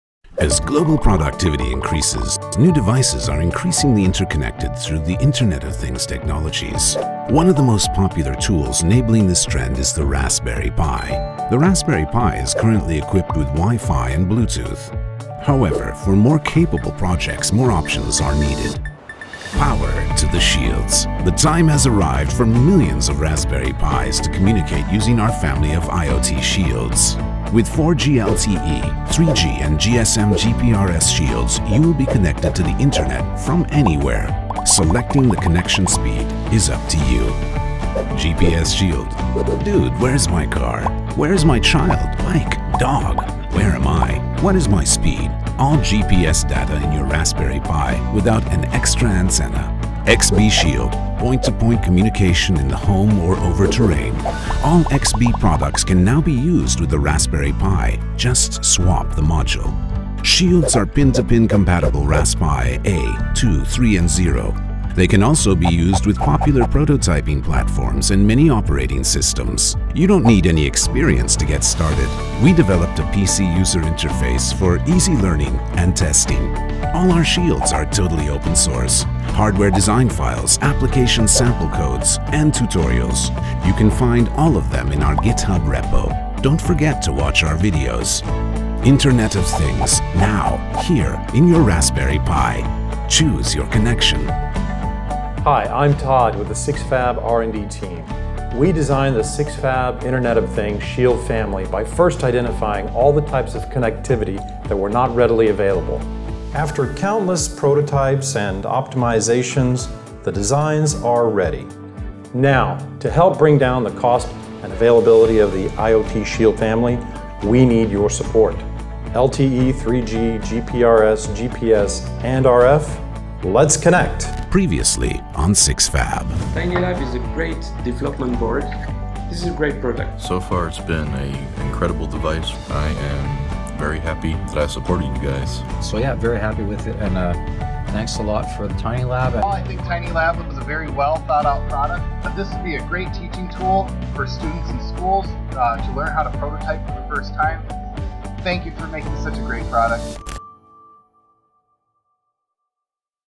Raspberry Pi Internet of Things Shield Family | The Most Affordable & Easy to Use
Visit us for IoT Shield Family and New products!

Perfect for entry into the world of IoT!

There is no experience required to enter the world of IoT since we provide a computer interface, sample applications, and open source codes of the entire project design. We've done our best for you to get connected, right out of the box.

With a 4G/LTE Shield, a 3G Shield, and a GSM/GPRS Shield, you will be connected to the internet from anywhere. Selecting the connection speed is up to you.

GSM GPRS Shield is our leading actor, because it's like an All-in-one! Have a need for speed? We offer connectivity up to 100 Mbps with our 4G/LTE Shield and 14Mbps with 3G Shield.

With our GPS Shield, all GPS data can easily be collected. Location, speed, and time can be used to derive much usedul information... With excellent stability and sensitivity the GPS shield consistently provides high signal reception efficiency.

The XBee Shield designed for XBee Modules by Digi allows all seemless connectivity with all XBee products.

Point to point communication up to 28 Miles, Wi-fi, Mesh network, in the home or over terrain, all at your fingertips.